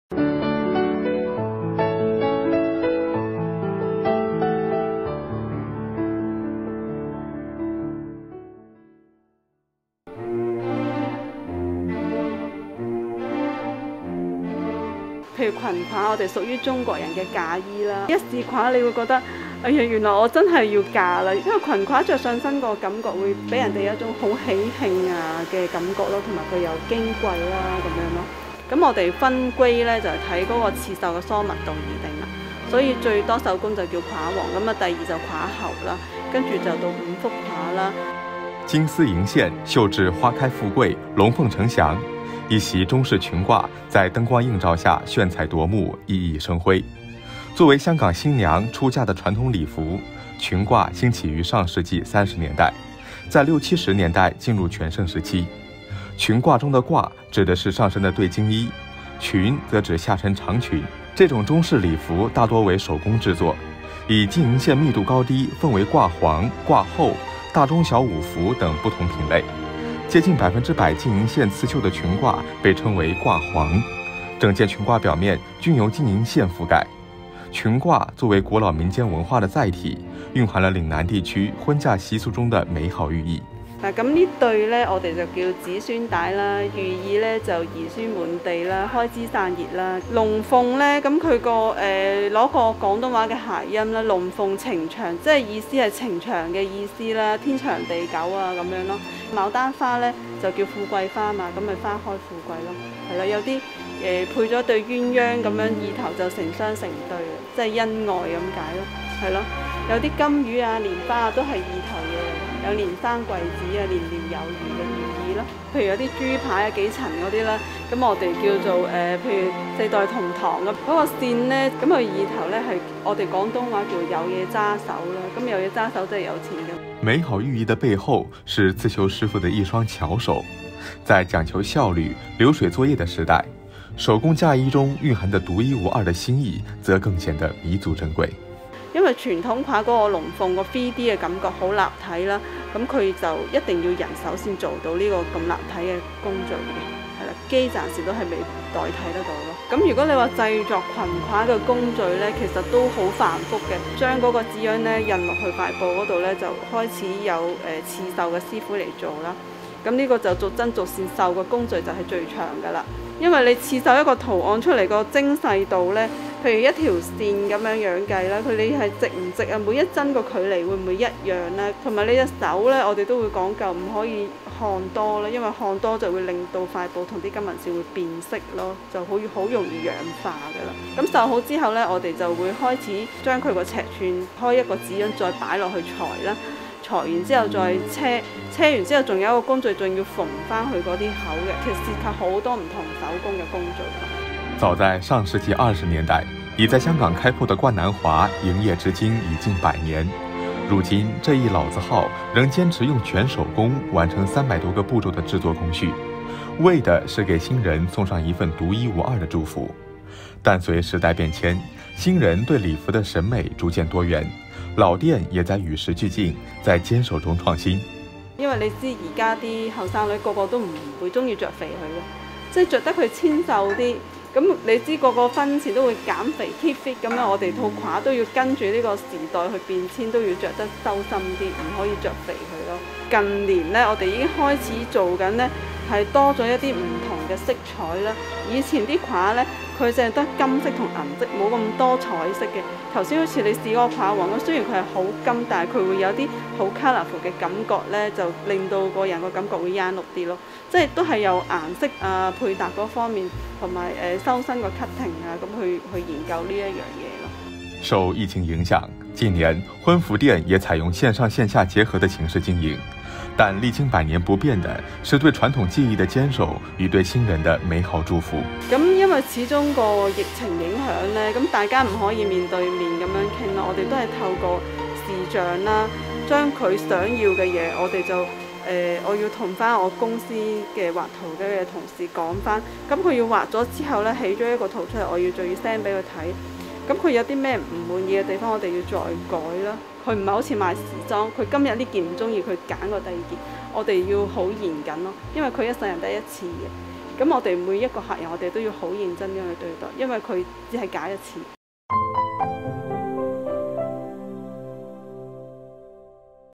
金丝银线绣百年婚俗 中式裙褂传续中创新
金丝银线，绣制花开富贵、龙凤呈祥，一袭中式裙褂在灯光映照下炫彩夺目、熠熠生辉。作为香港新娘出嫁的传统礼服，裙褂兴起于上世纪30年代，在六七十年代进入全盛时期。裙褂中的“褂”指的是上身的对襟衣，“裙”则指下身长裙。这种中式礼服大多为手工制作。以金银线密度高低分为褂皇、褂后、大中小五福等不同品类。接近100%金银线刺绣的裙褂被称为“褂皇”，整件裙褂表面均由金银线覆盖。裙褂作为古老民间文化的载体，蕴含了岭南地区婚嫁习俗中的美好寓意。